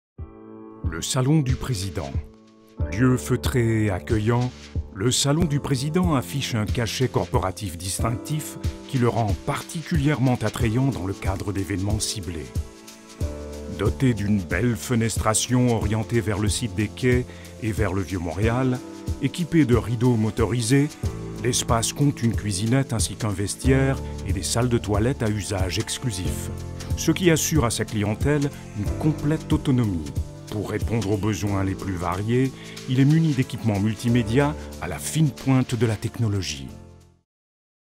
Salon du président - Location de salles
Particularités

    * Espace qui inclut ses propres installations, dont une cuisinette, un vestiaire et des toilettes à usage exclusif.
    * Entièrement équipé en multimédia.

Superficie

    * 115 m2 (1238pi2)
    * Longueur : 12,5 m (40 pi 11 po)
    * Largeur : 8,2 m (26 pi 10 po)
    * Hauteur des plafonds : 3,4 m (11 pi)

Capacité

    * Banquet : 48
    * Cocktail : 90
    * Réunion : 22
    * Théâtre : 72

Capacité électrique

    * 5 circuits 15A  120V